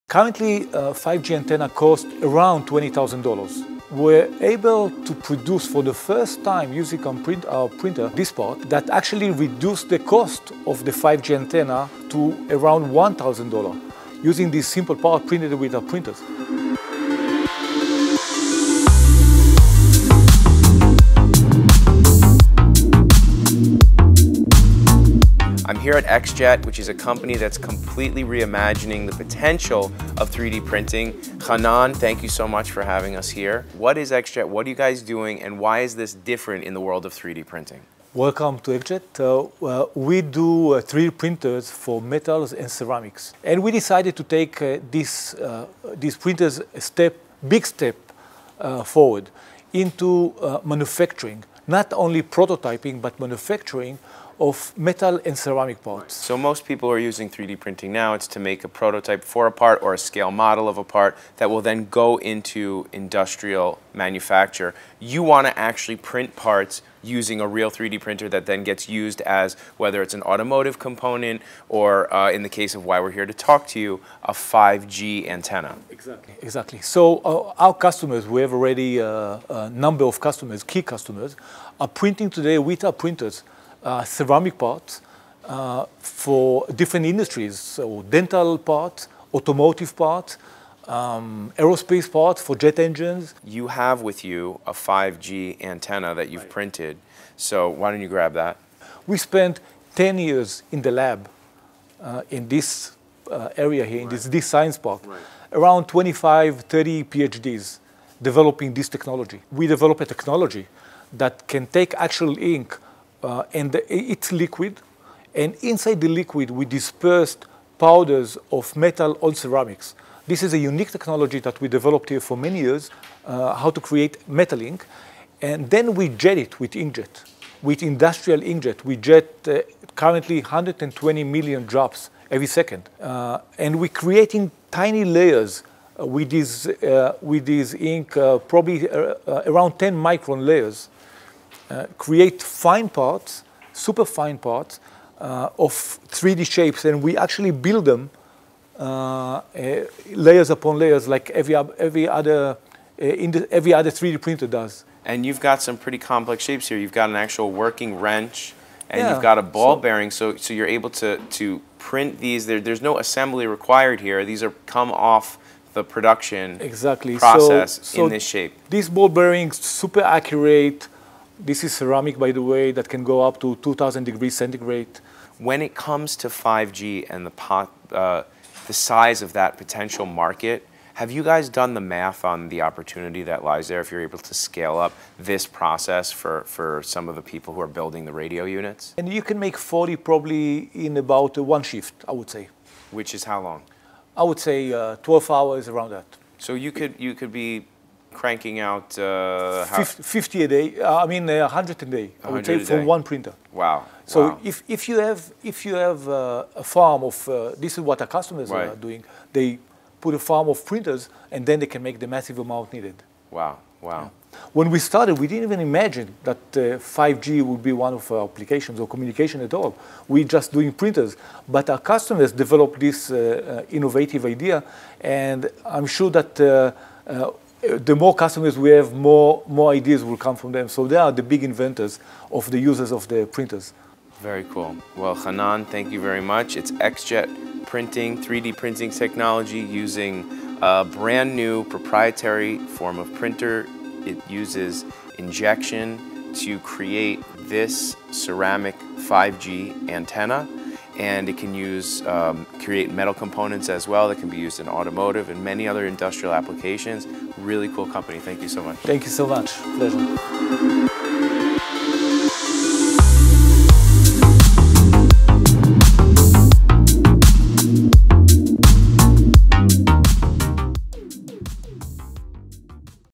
5G4REAL: These 3D-Printed 5G Antennas Are Cheap & Ready to Install
The expansion of 5G networks will require an unprecedented increase in the number of radio units, especially in urban environments.